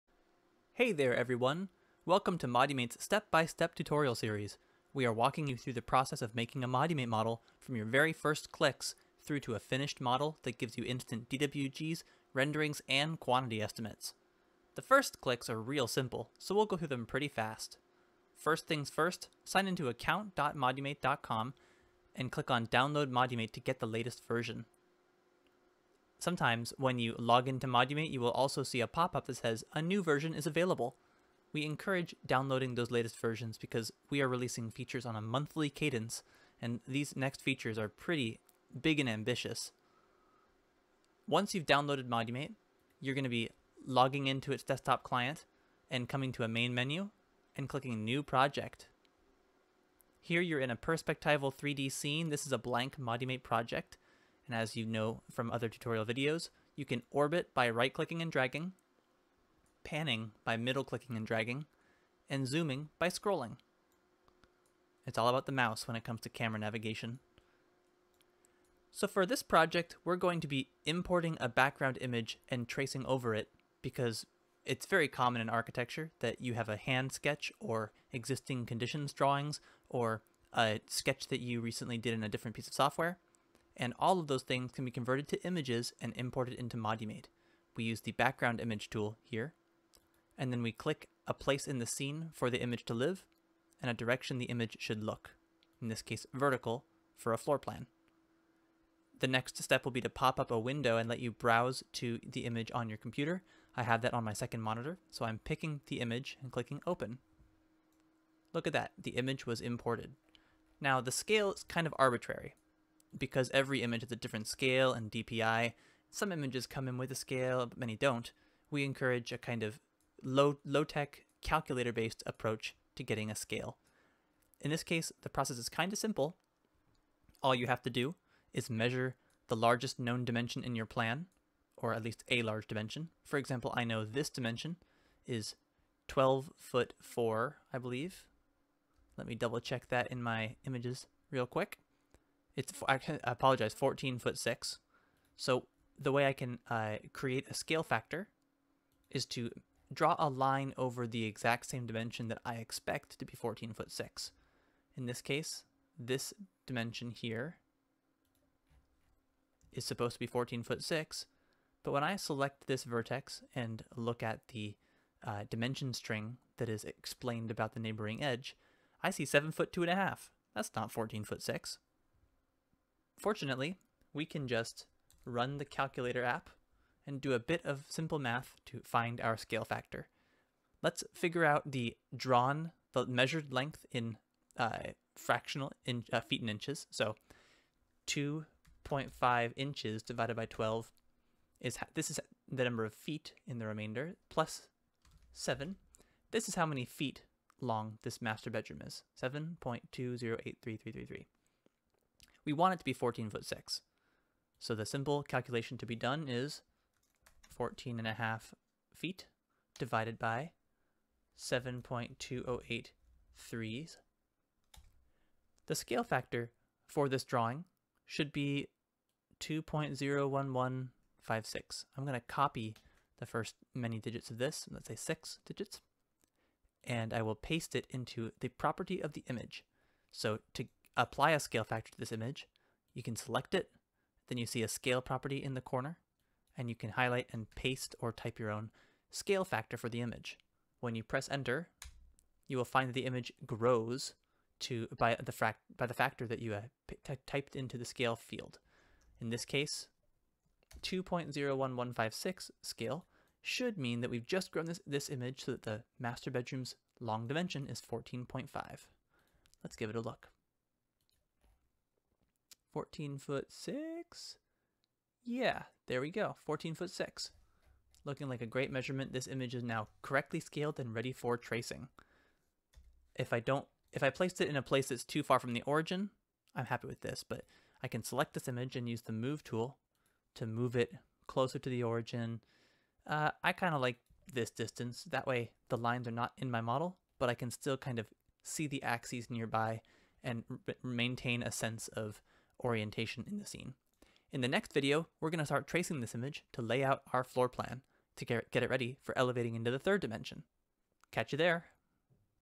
Stage 1 (Massing), Step 1: Start A New Project / Import Image
In this video, you'll learn how to start a new project, and import a background image for tracing.